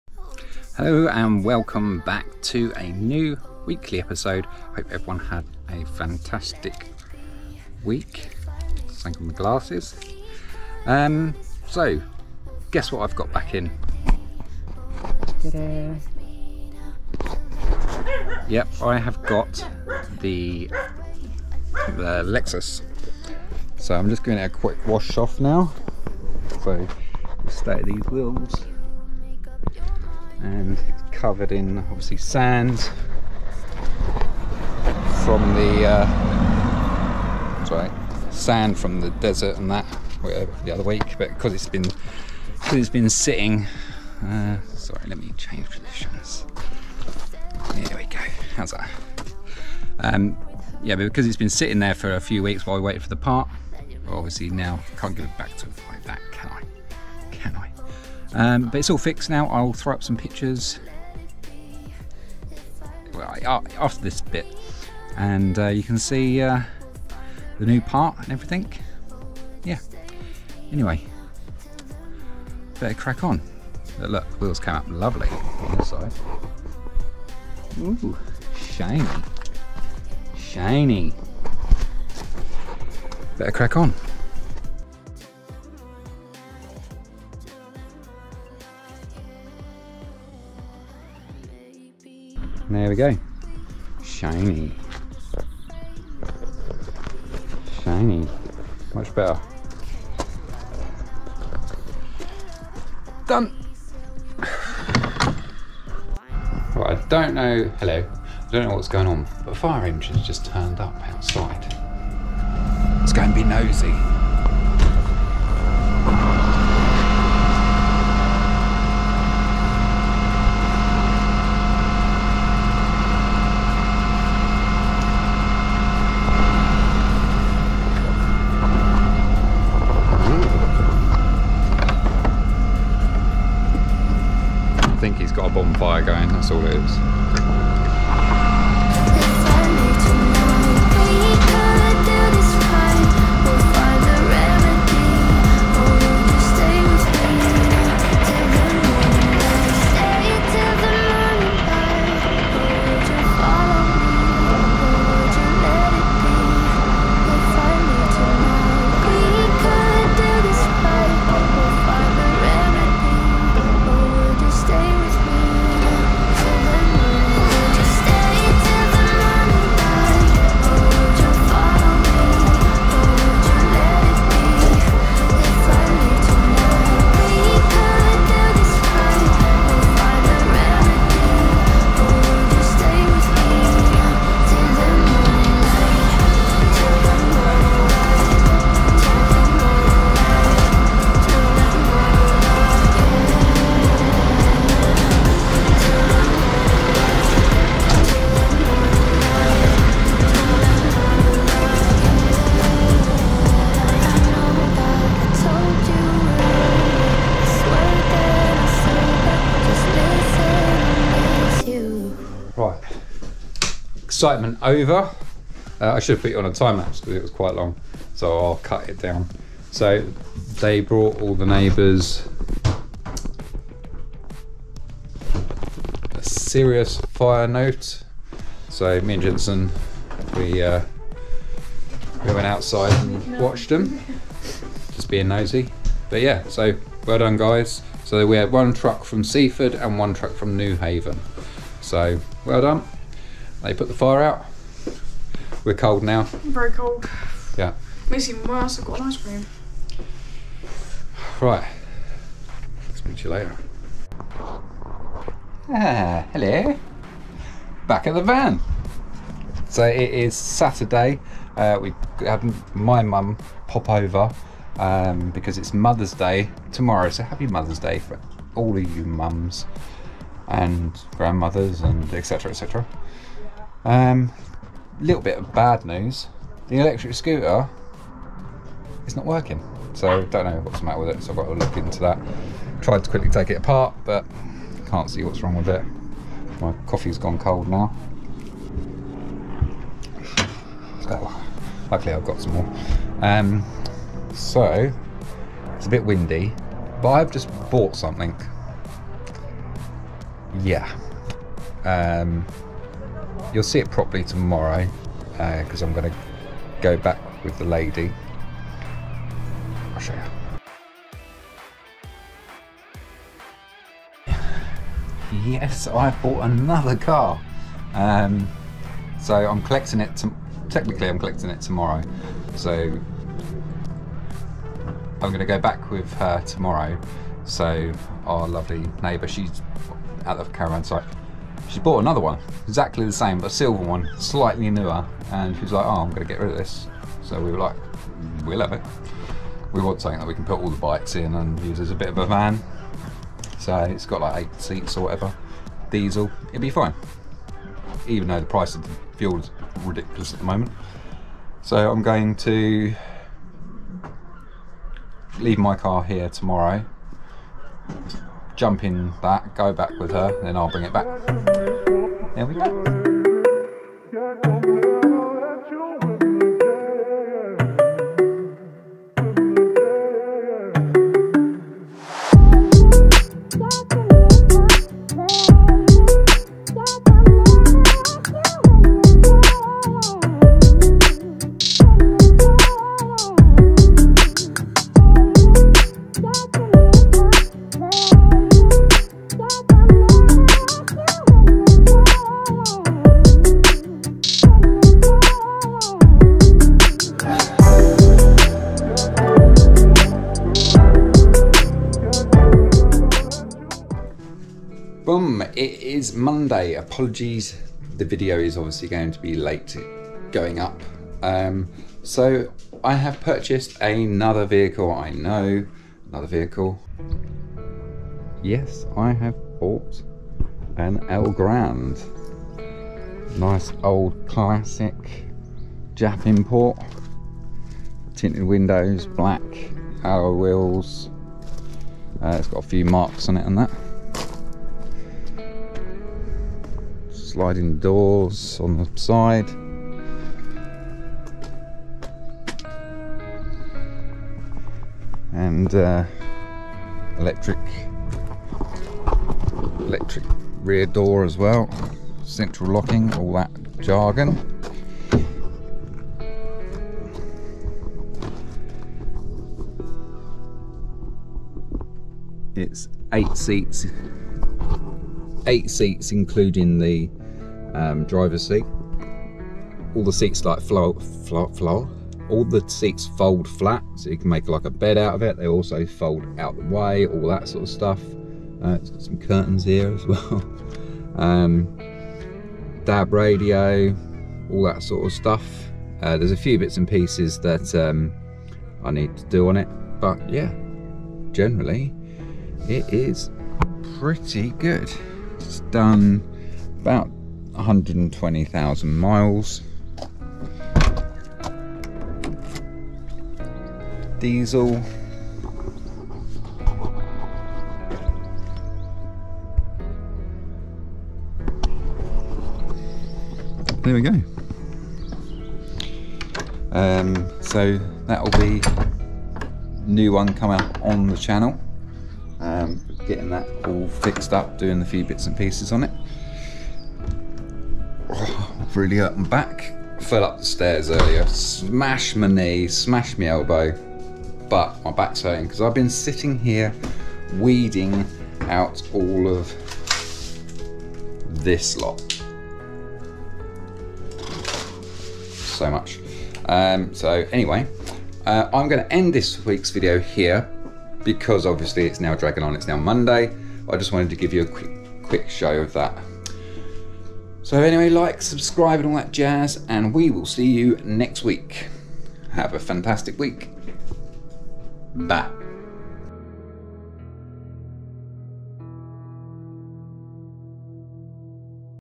A fire at the neighbours /Our Week Episode 64
Hello everyone and welcome back to a new video on this week's episode the neighbour has a fire and the fire brigade have to come out and I bought another car.